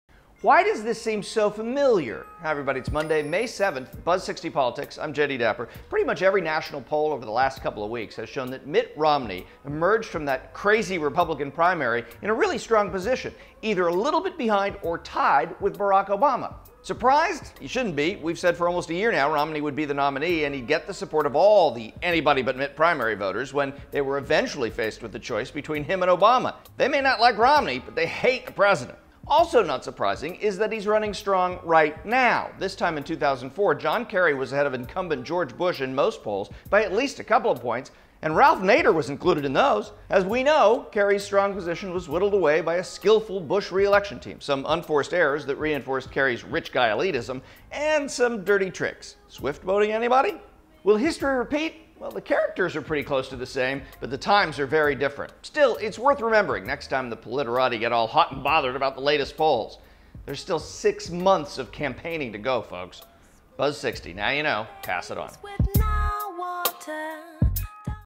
Romney and Obama Tied in Most Polls, Like Kerry and Bush: Buzz Politics 5.7
National polls released in the last two weeks show Mitt Romney either slightly behind or tied with Barack Obama, indicating the presumptive Republican nominee has weathered the divisive primary better than many predicted. But it's no surprise to us and it may not mean much either if Kerry-Bush 2004 polls are any guide.